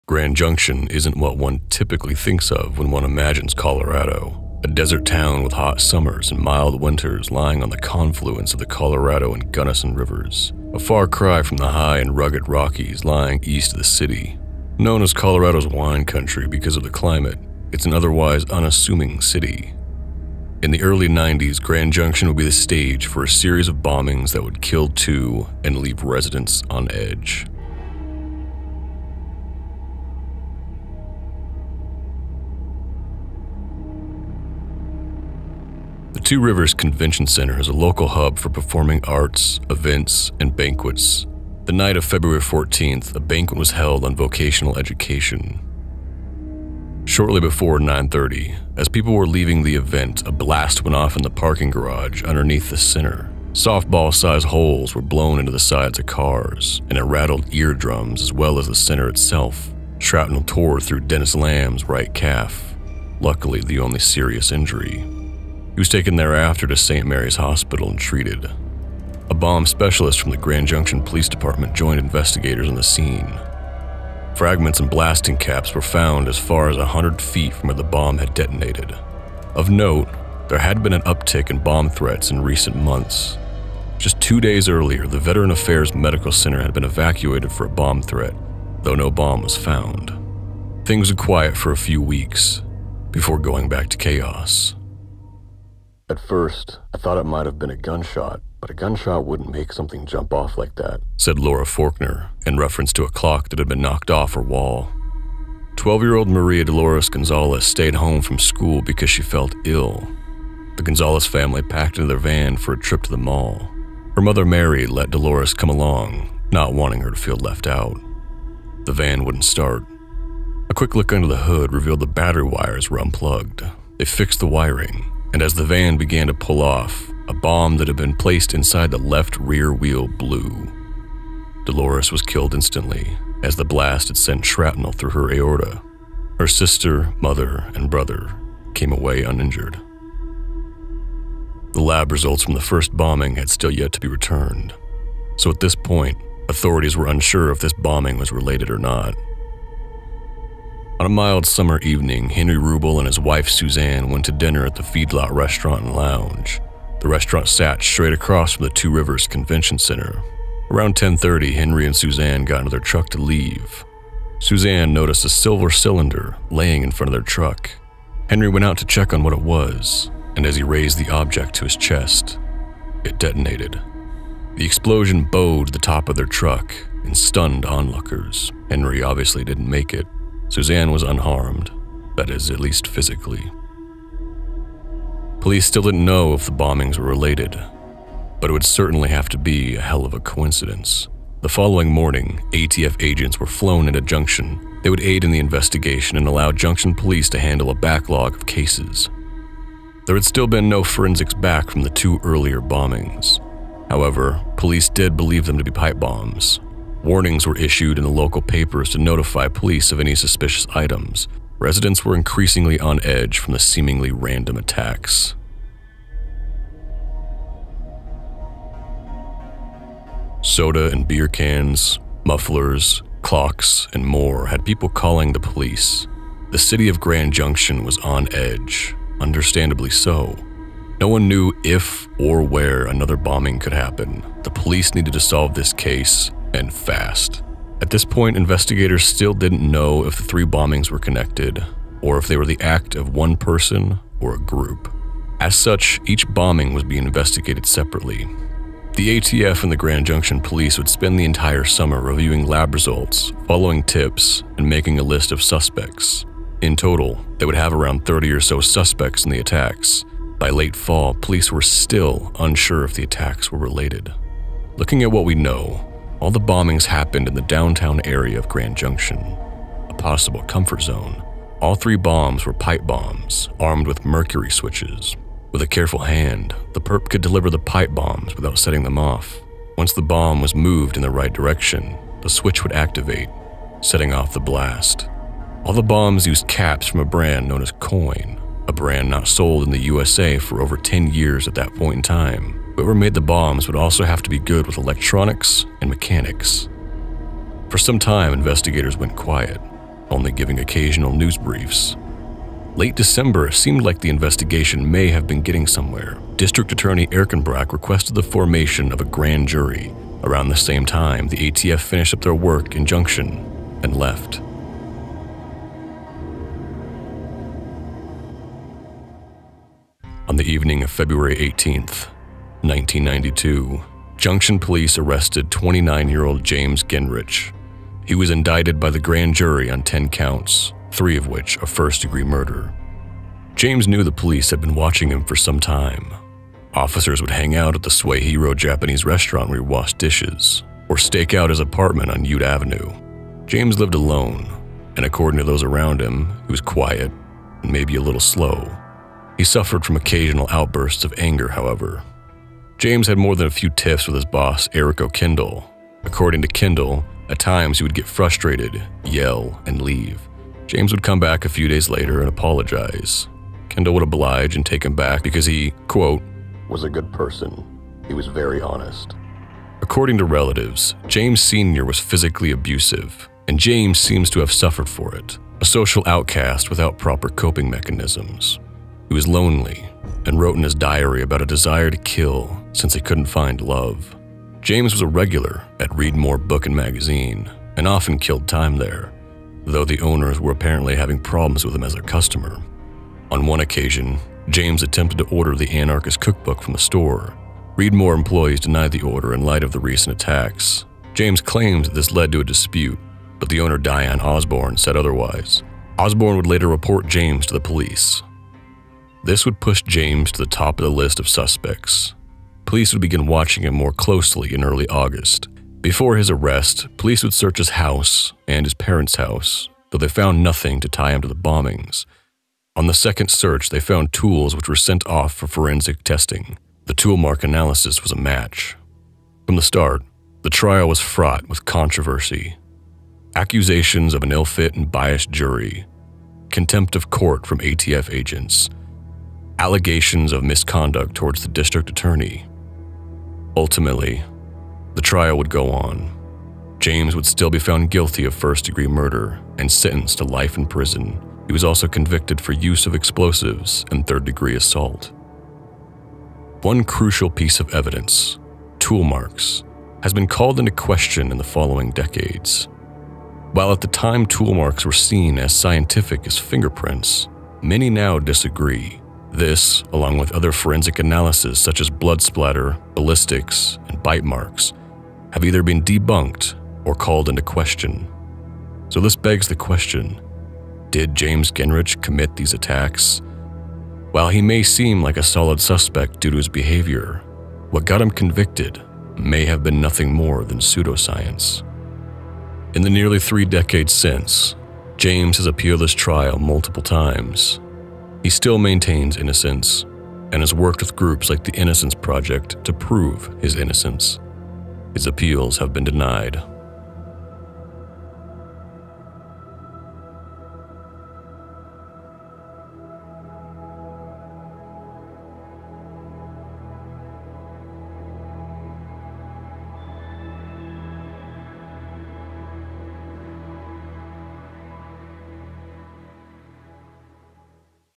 Trail Of Destruction - The 1991 Grand Junction Bombings
In 1991 a series of bombings would strike the town of Grand Junction, Colorado